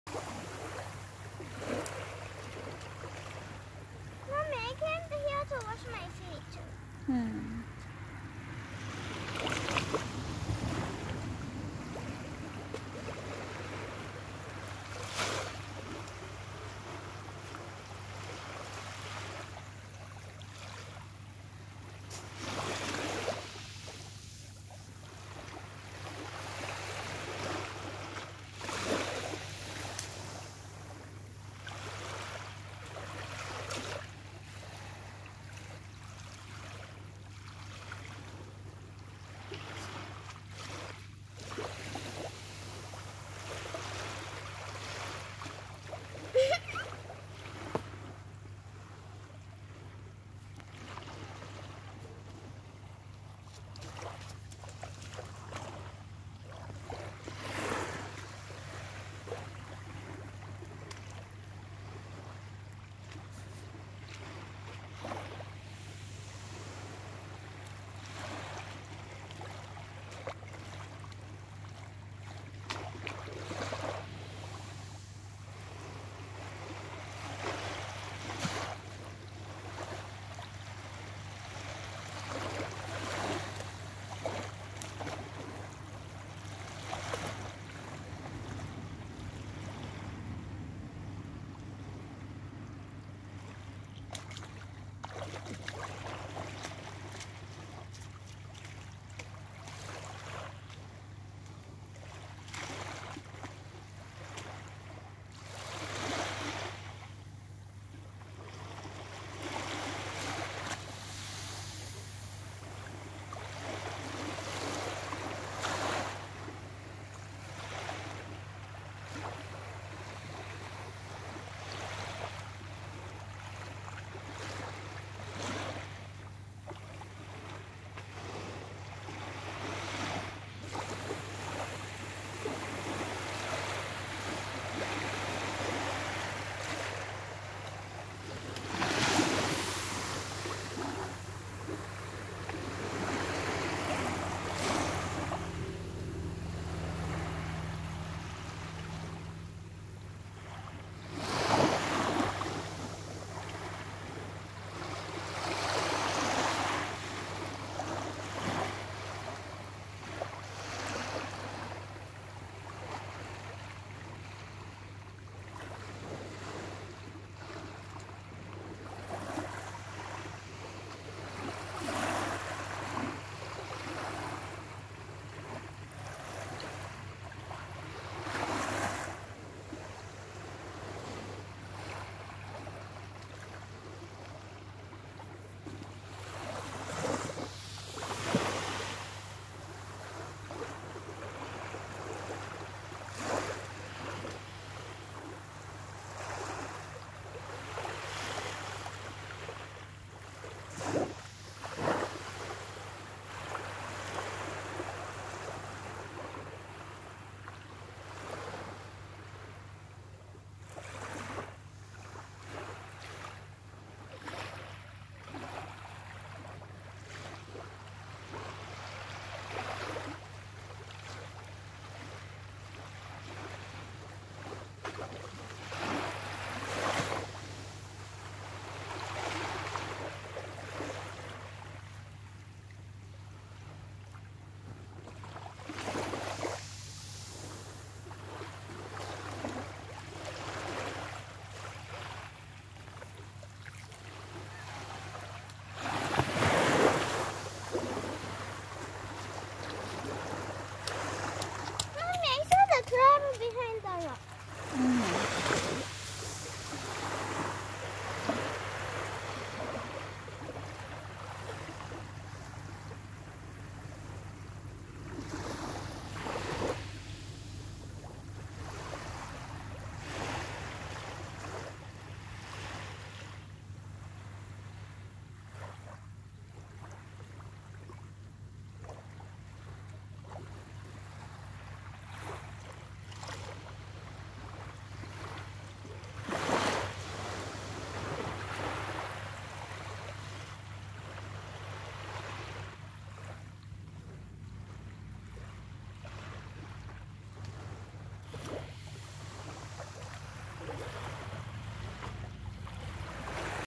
Relaxing Early Morning Beach
Relaxing music
Nature
The natural beach of Addu City, Maldives